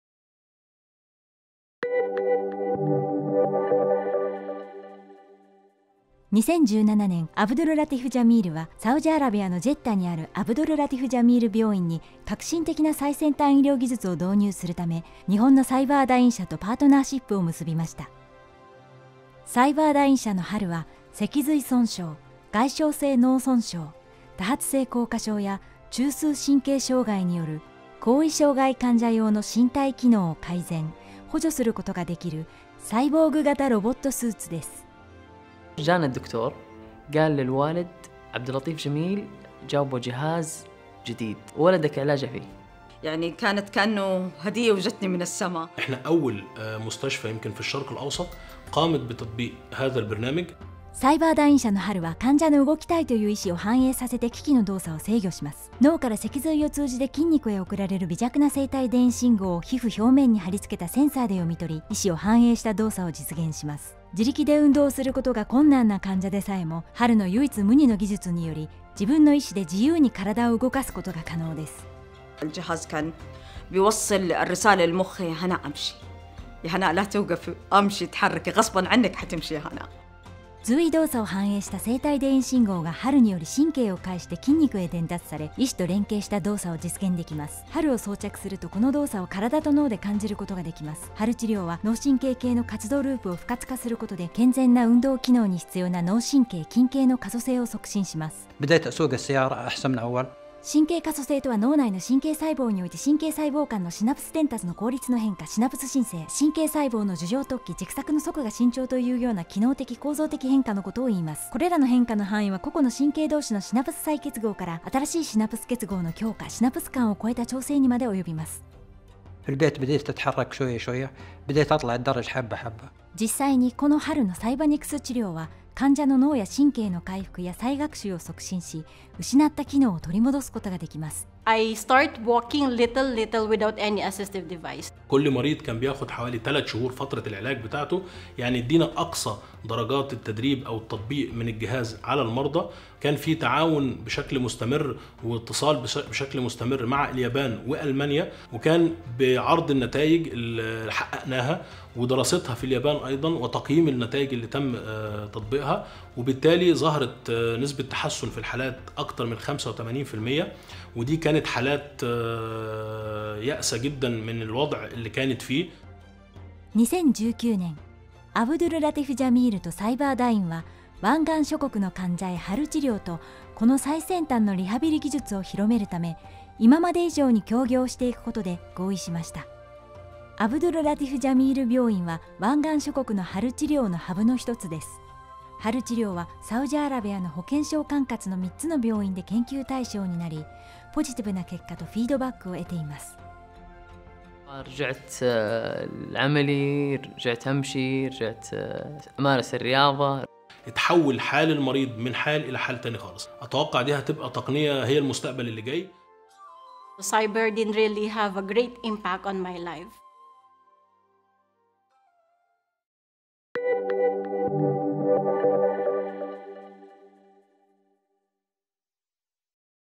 Cyberdyne at the Abdul Latif Jameel Hospital (JP VO Subs 16x9)
In 2017, Abdul Latif Jameel partnered with Japanese innovator Cyberdyne, to bring their breakthrough technology to Saudi Arabia at the Abdul Latif Jameel Hospital in Jeddah.

The Cyberdyne Hybrid Assisted Limb or HAL®, is a unique exoskeleton therapeutic device used for treatment of patients with physical disabilities caused by spinal cord injuries, traumatic brain injuries, multiple sclerosis, and other diseases of the central nervous and neuromuscular systems.

In 2019, Abdul Latif Jameel and Cyberdyne expanded their collaboration to increase accessibility to cutting-edge innovative spinal injury rehabilitation technology to patients across the Gulf region, and Abdul Latif Jameel Hospital became the GCC regional training hub.